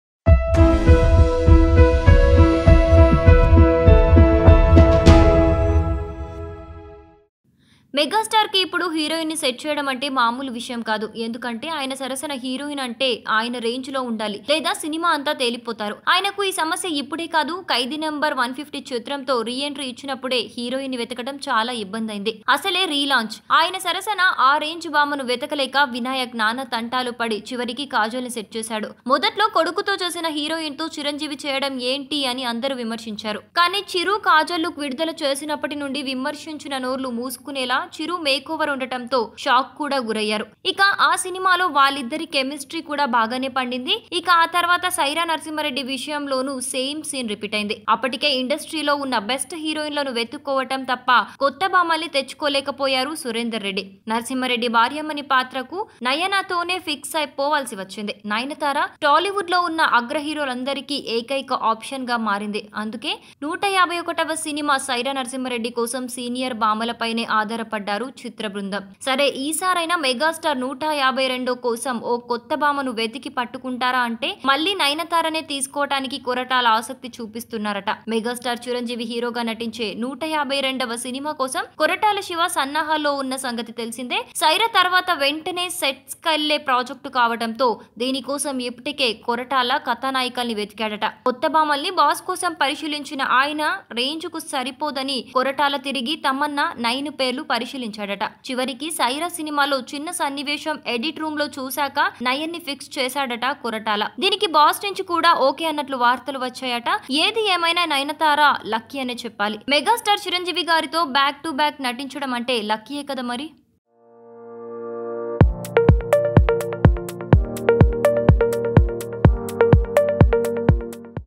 Mega Star Chiranjeevi To Pair Up With Nayantara | Tollywood Updates | Telugu Full Screen
Mega Star Chiranjeevi To Pair Up With Nayantara. Sye Raa Narasimha Reddy Movie Is Directed By Surender Reddy And Produced By Ram Charan. Music Composed by Amit Trivedi. Chiranjeevi, Amitabh Bachchan, Sudeep, Vijay Sethupathi, Tamannaah Bhatia, Nayantara, Brahmaji, Jagapati Babu Are Playing Main Lead Roles In This Movie. Watch Latest Videos And For More Updates.

ENTERTAINMENT

MOVIES

NEWS